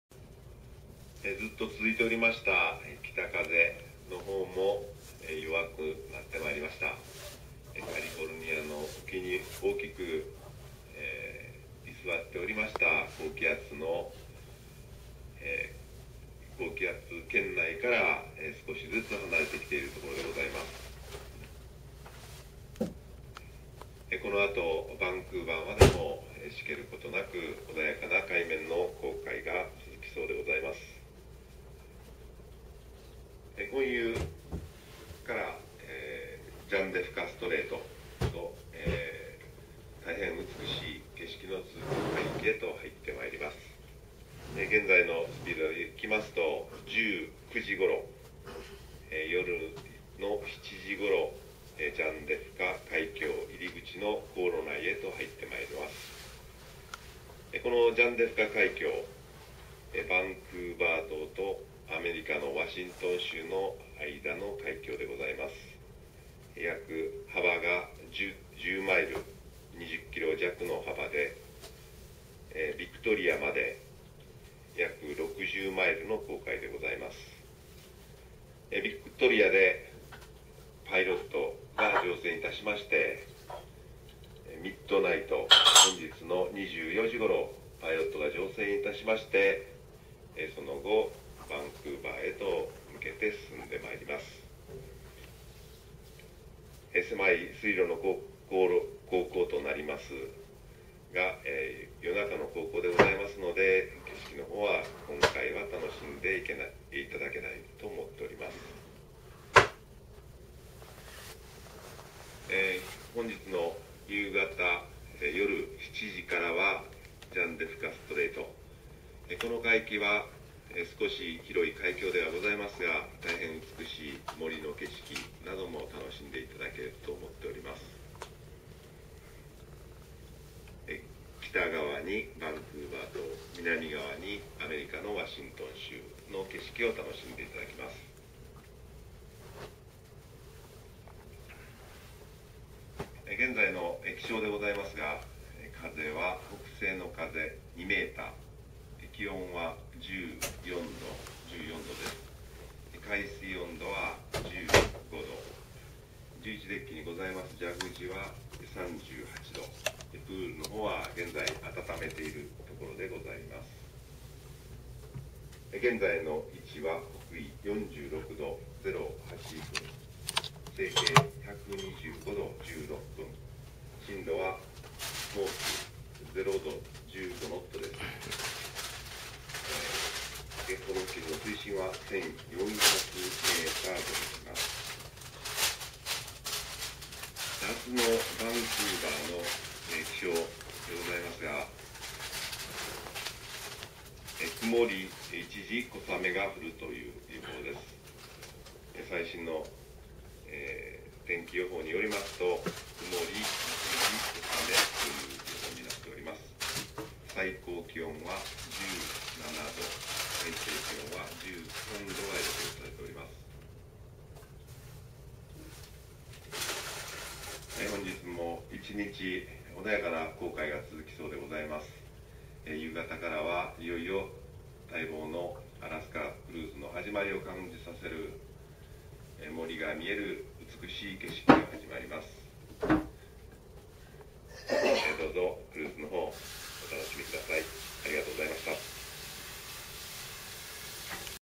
世界一周クルーズファンデスカ海峡沖終日航海
2010年飛鳥Ⅱ世界一周クルーズにて、ファンデスカ海峡沖合終日紅海 ※世界一周クルーズ旅行記はこちらのアドレスでご覧ください。